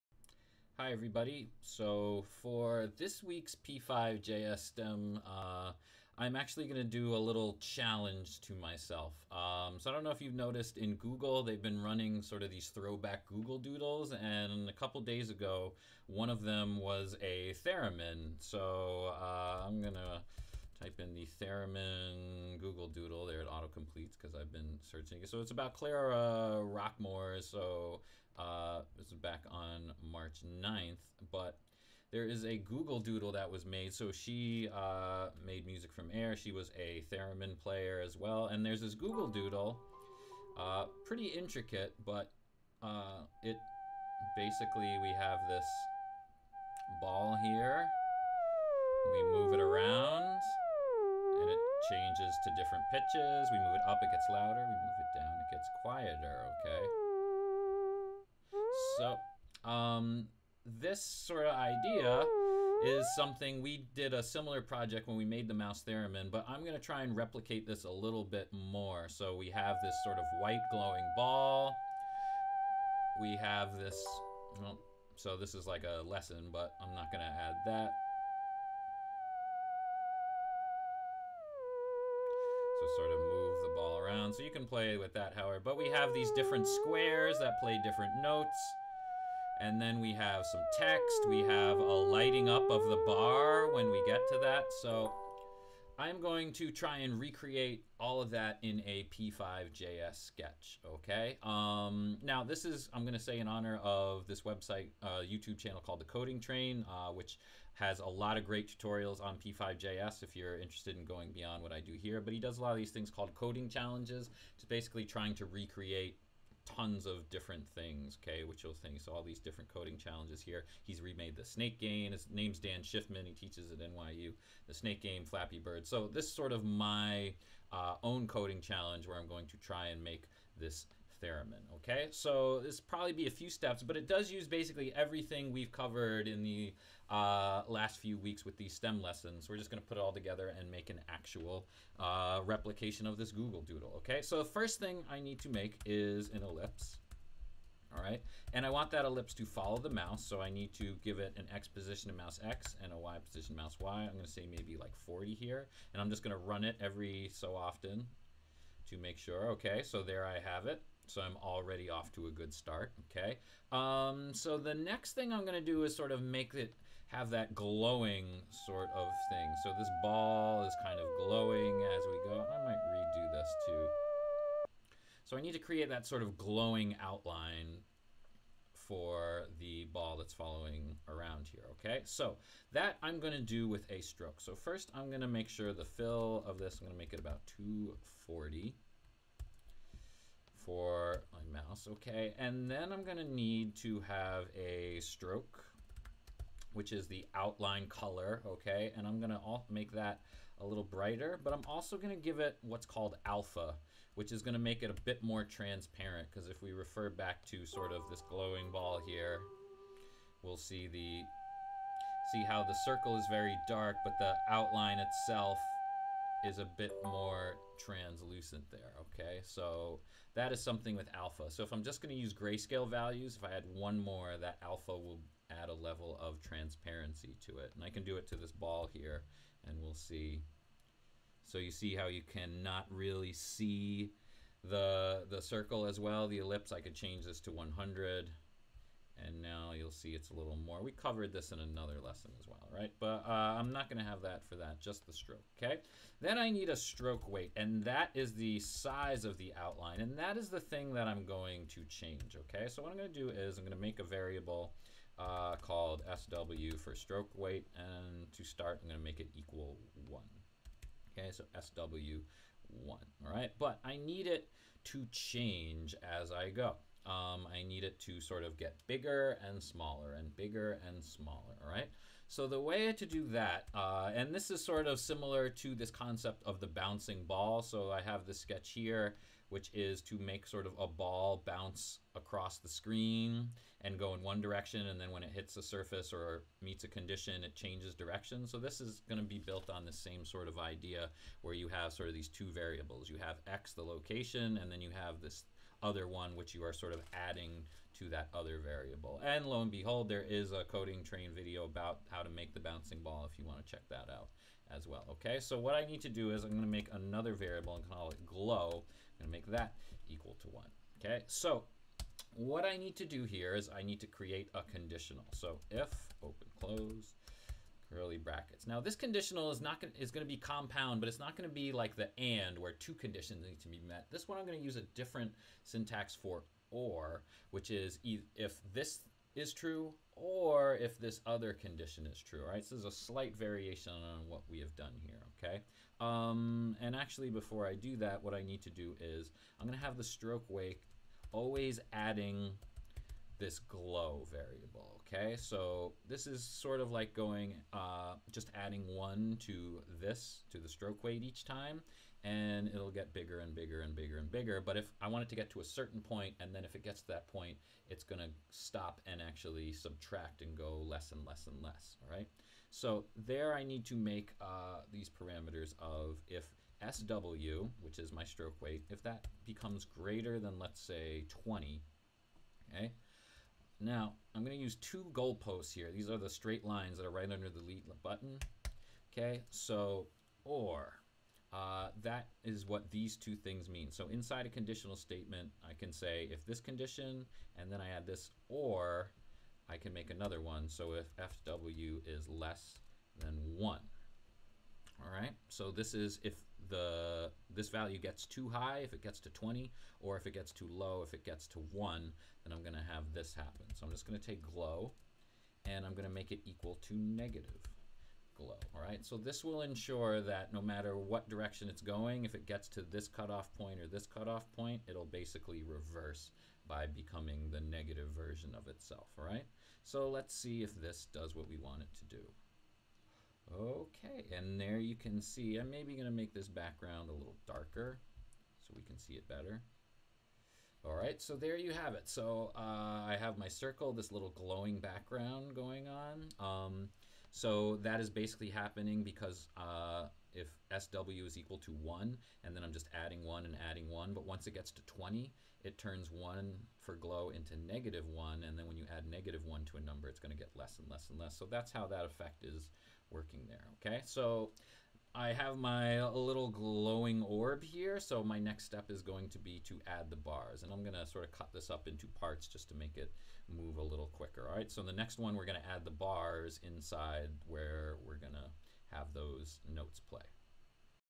p5.js Tutorial - Recreating Theremin Google Doodle part 1: Glowing Orb
This project uses p5.js to recreate the Theremin Google Doodle for Clara Rockmore's 105th Birthday

This video starts by making a glowing orb that moves with the mouse. It also introduces the concept of 'or' syntax in a compound conditional statement.

These videos were made during quarantine for a STEAM class I teach, so there may be references to the class or an assignment.